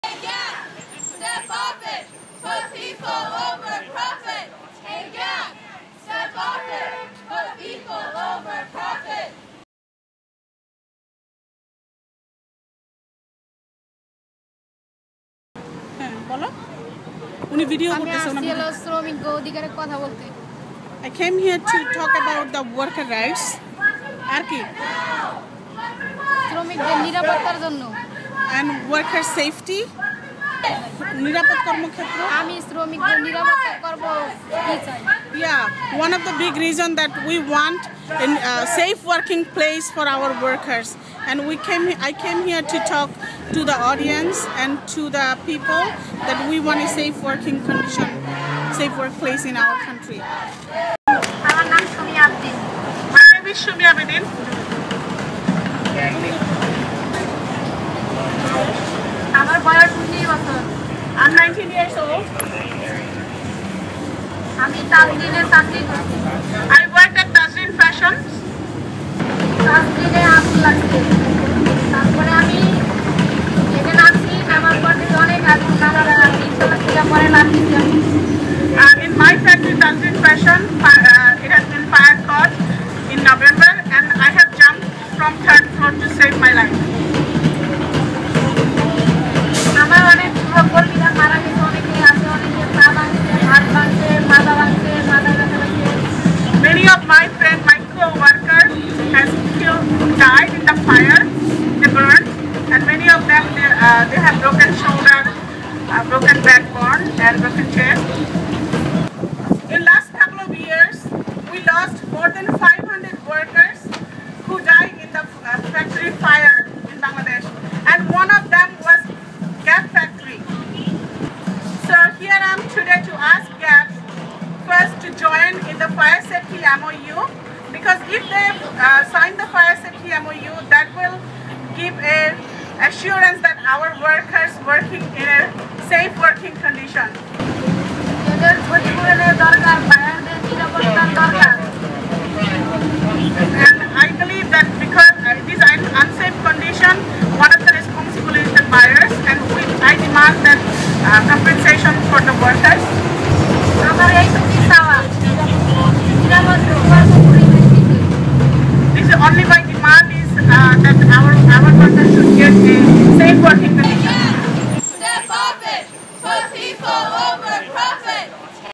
Bangladesh Fire Survivor
Seamstress Sumi Abedin survivor of the factory fire in Bangladesh on November 24, 2012 tours the US calling for worker compensation from the main buyers, GAP, Wal-Mart, and Disney. Here she is at the GAP in Hollywood California